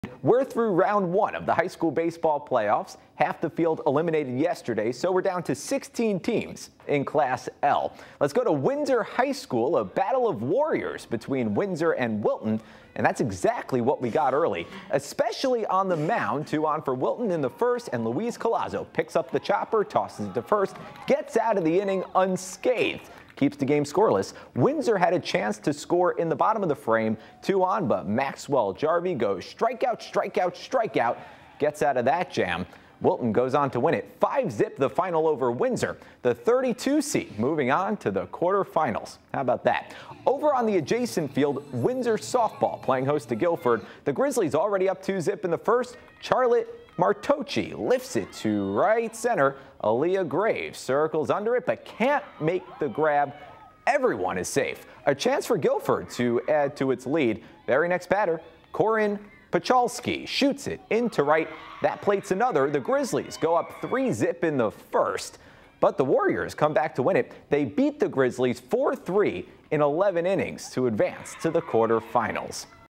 Wilton baseball, Windsor softball advance in State playoffs
Wilton won 5-0 to advance to the quarterfinals and Windsor won 4-2 in extra innings to advance to the quarterfinals.